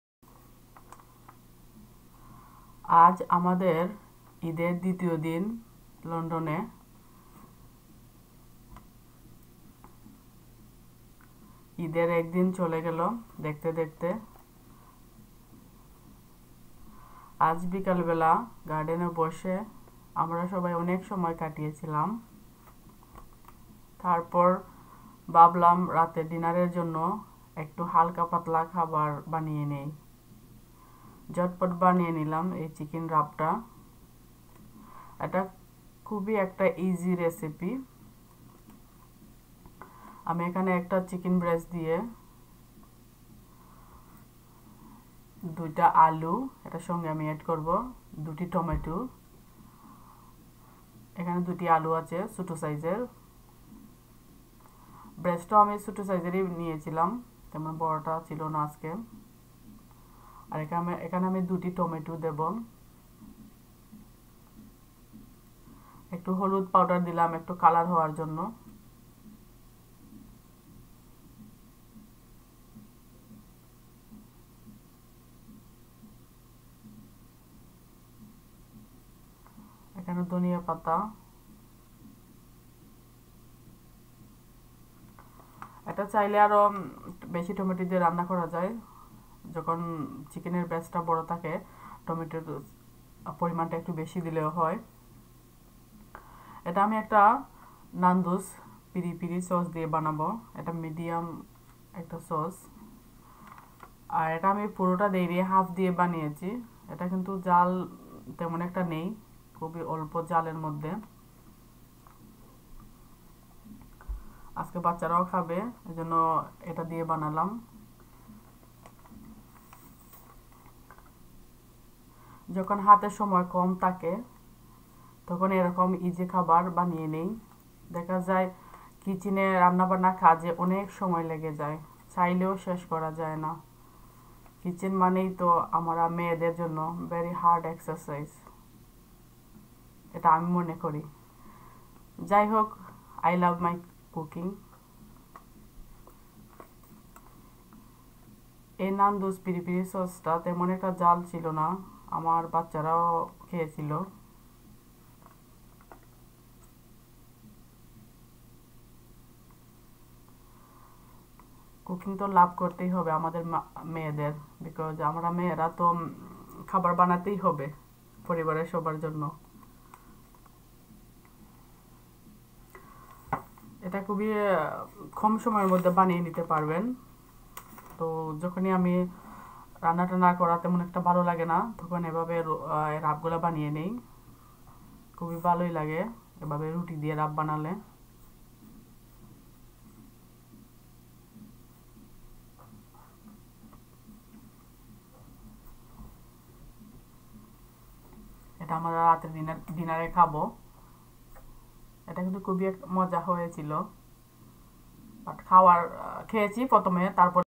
Eid এর Next day ঘরে কি কি হয়েছিল
Eid এর  Next day  Nando's Chicken  Wrapবানালাম One Chicken breast Two Tomato two potato one Onion Cooking Oil  Nando's peri peri Sauce Medium some coriander  Haldi Ground Turmeric And Salt.